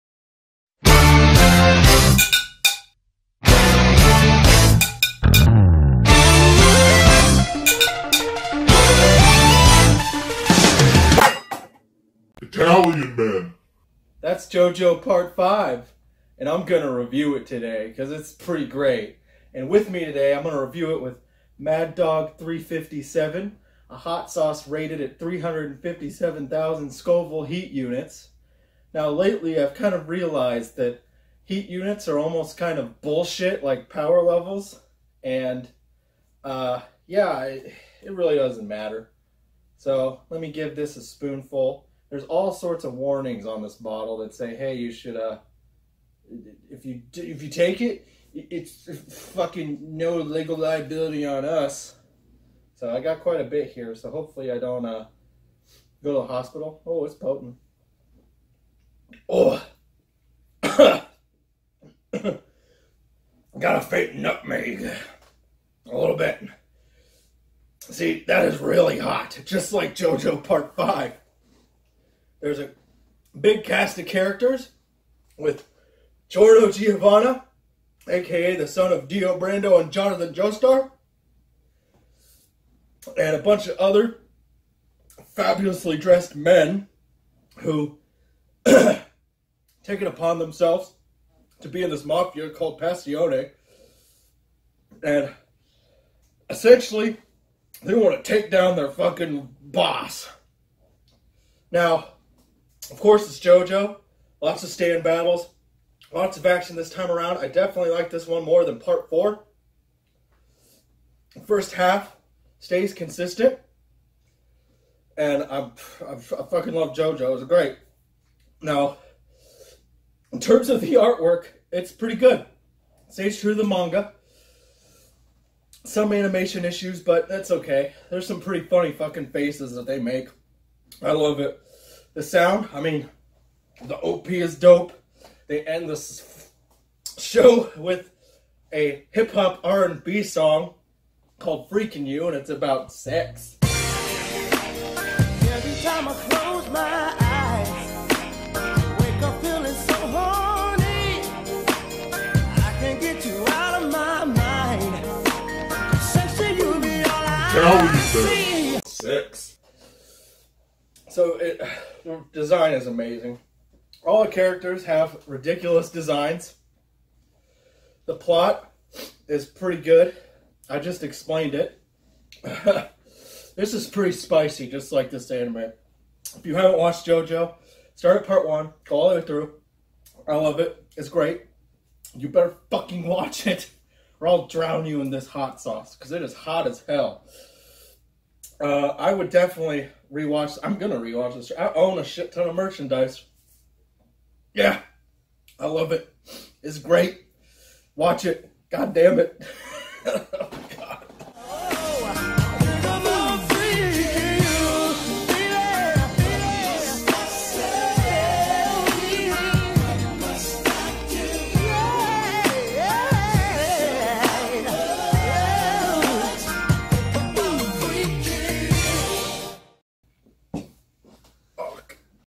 JoJo's Bizarre Adventure Vento Aureo Season 1 REVIEW feat. Mad Dog 357 Hot Sauce
Just watch the show already.

SHOW: JoJo's Bizarre Adventure: Vento Aureo
SAUCE: Mad Dog 357